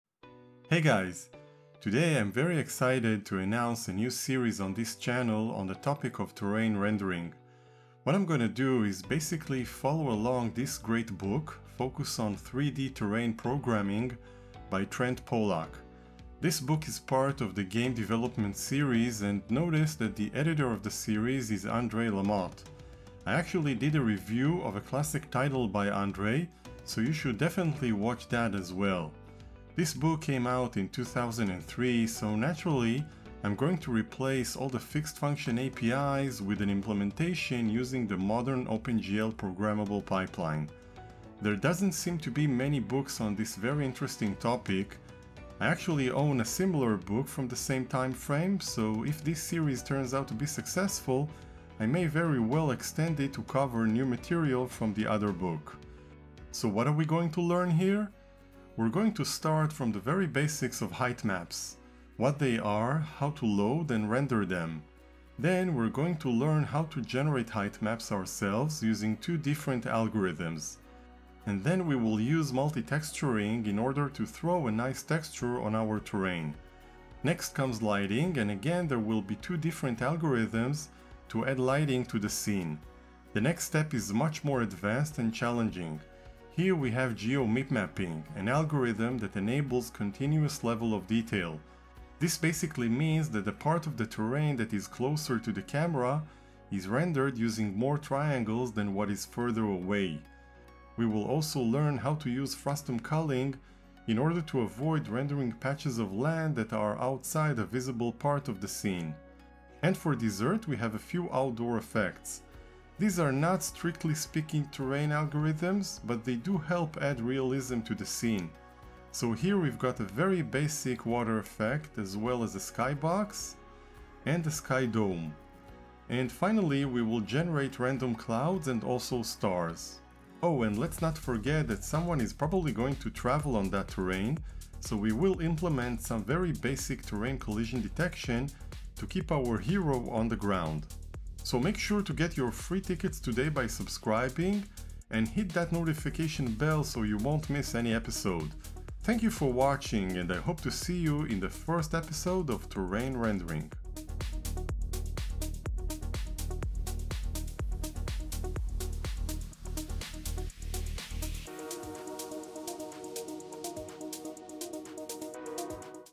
Announcing The 'Terrain Rendering' Series // Modern OpenGL
This is an announcement video for an upcoming series on Terrain Rendering. In this series we will follow along "Focus on 3D Terrain Programming" by Trent Polack. We will develop algorithms for procedurally generated heightmaps, add terrain lighting and texturing, continuous level of detail using geomipmapping and also outdoor effects such as simple water, skybox and skydome.

🔥 See the list of the books that I'm using as background information for my tutorials I highly recommend them for learning more about OpenGL and Vulkan.

to see more of my tutorials on modern OpenGL.

Etay Meiri